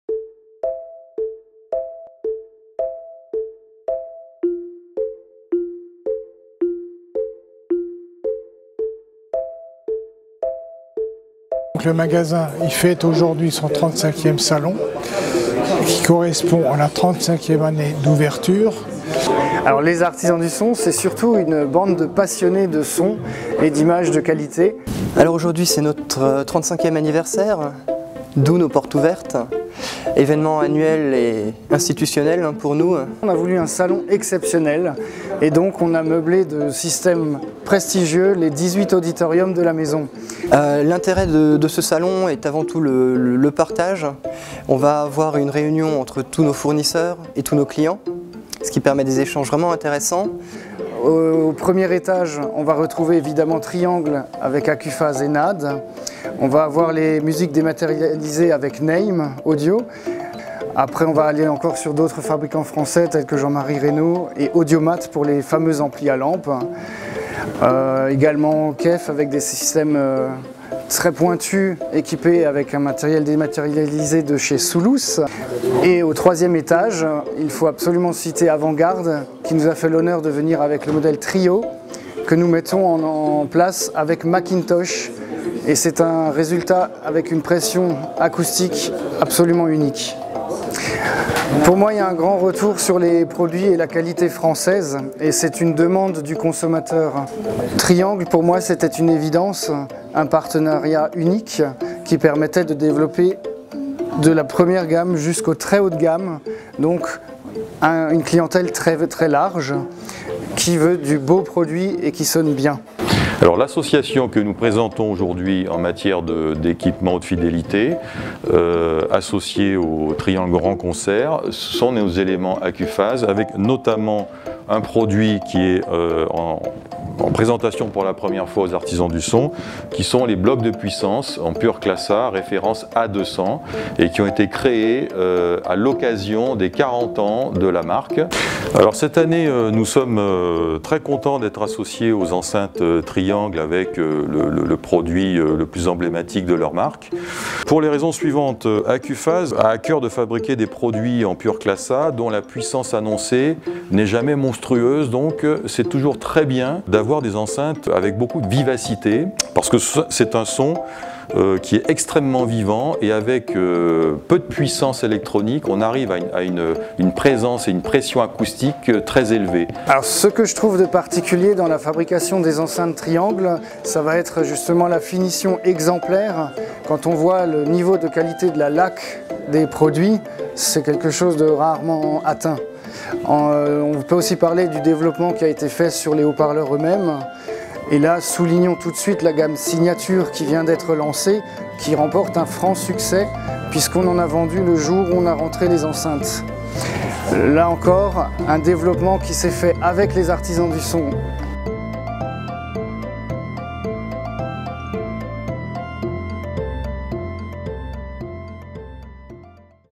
Les Artisans du Son 2013 - Triangle HIFI
Triangle était présent à l'anniversaire des 35 Ans des Artisans du son, magasin culte situé à Mulhouse.  Robert Rapp et sa famille ont crée il y 35 ans un magasin qui est maintenant une référence par son  accueil chaleureux, sa grande qualité de conseils et ses 18 auditoriums réunissant  les marques de prestige de la Haute Fidélité.

Triangle voulait mettre en avant tout ces hommes qui travaillent chaque jour à faire découvrir le plaisir et de la Hifi et de la musique dans toute la France. Les artisans du son est une belle histoire qui n'est décidément pas près de s'arrêter.